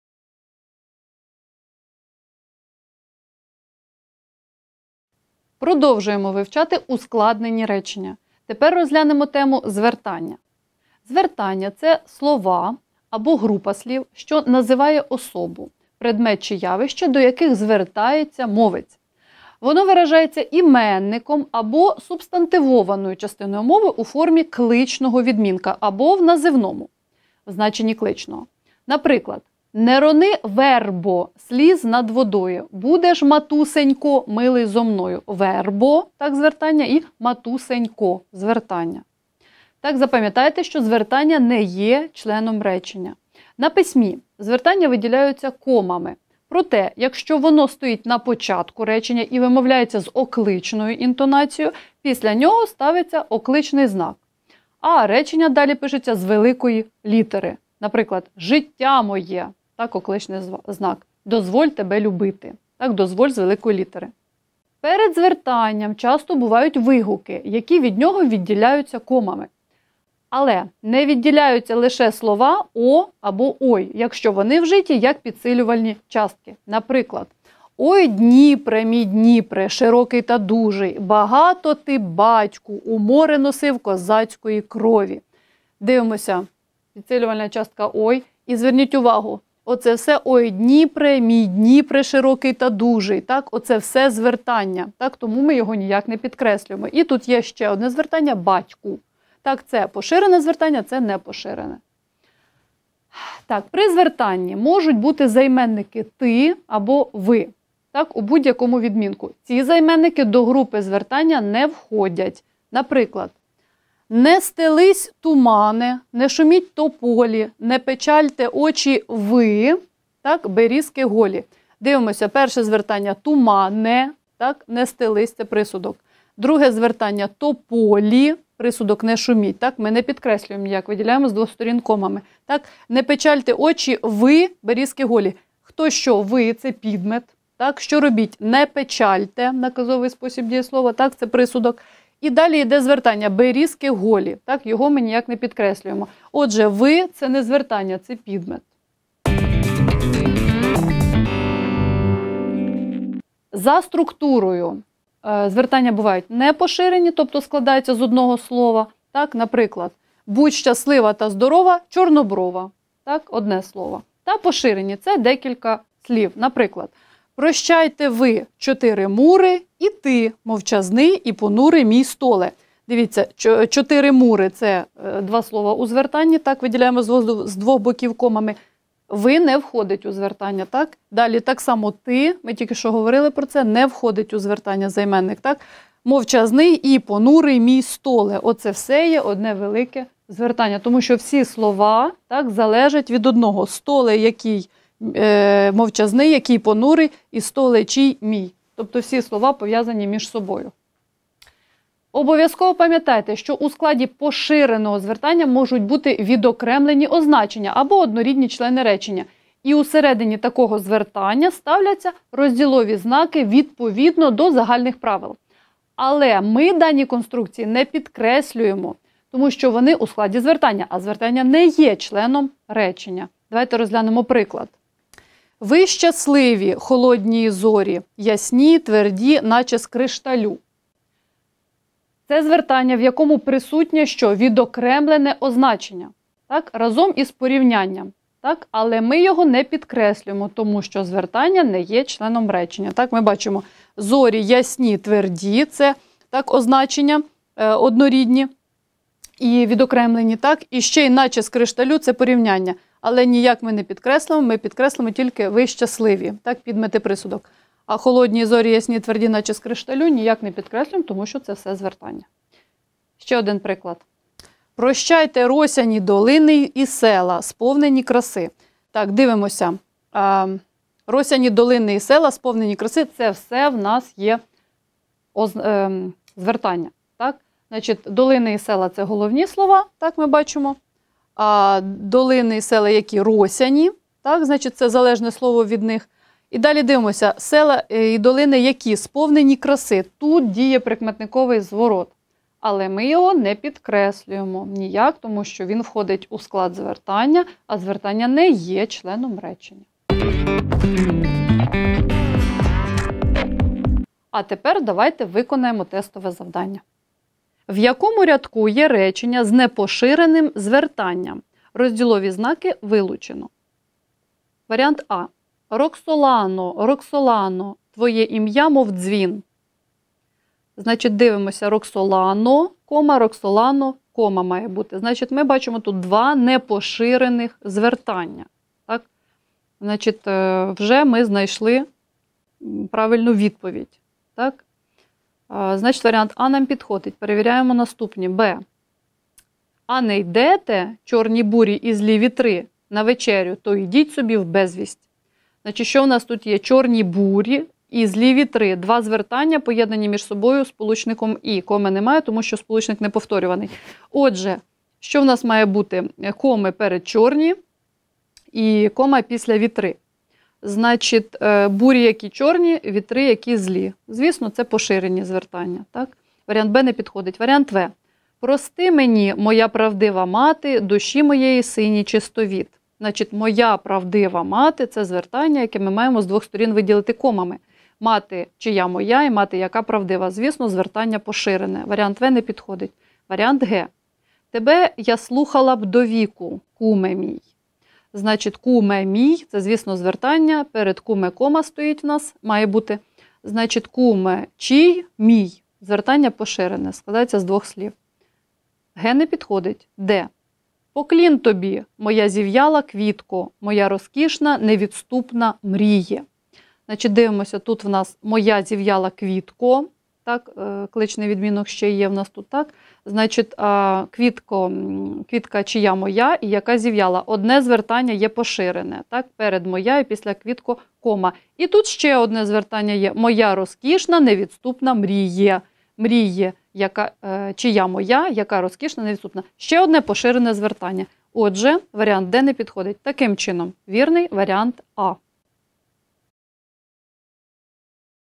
ЗНО. Укр.мова. 17.3. Речення зі звертанням. Звертання непоширені й поширені
Речення зі звертанням. Звертання непоширені й поширені. Розділові знаки при звертаннях